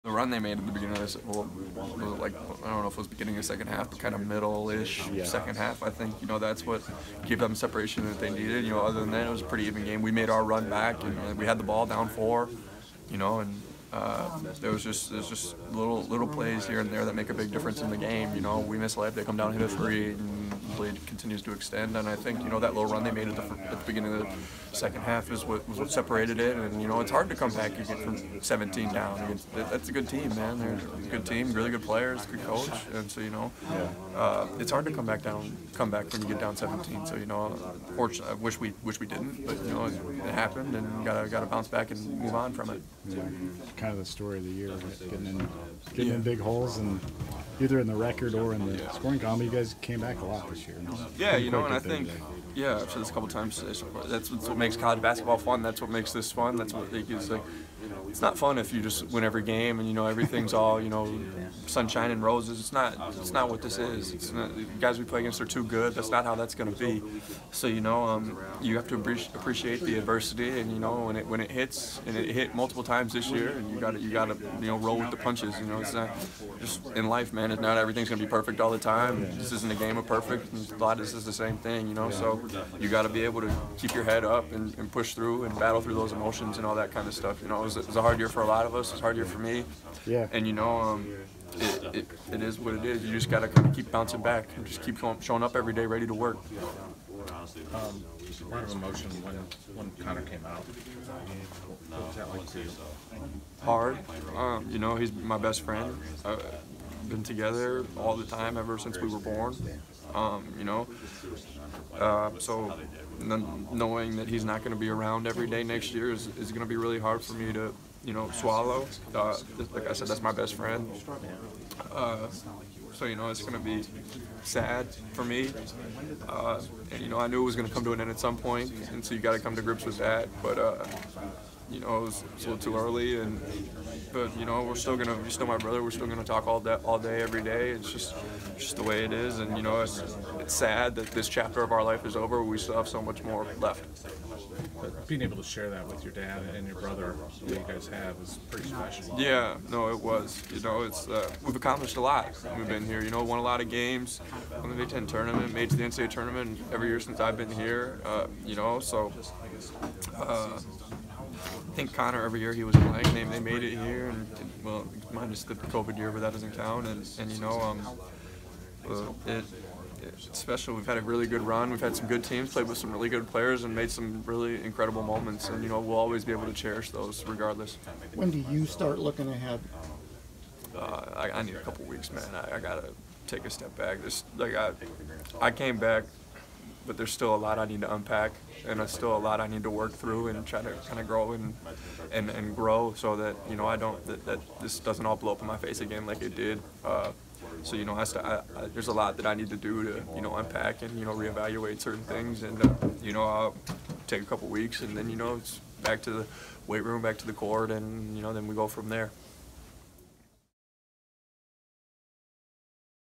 Patrick McCaffery: 'It was a hard year for a lot of us'
Iowa basketball's Patrick McCaffery was emotional after Iowa's 83-75 loss to Auburn.

🎥: Chad Leistikow  (@ChadLeistikow  on Twitter)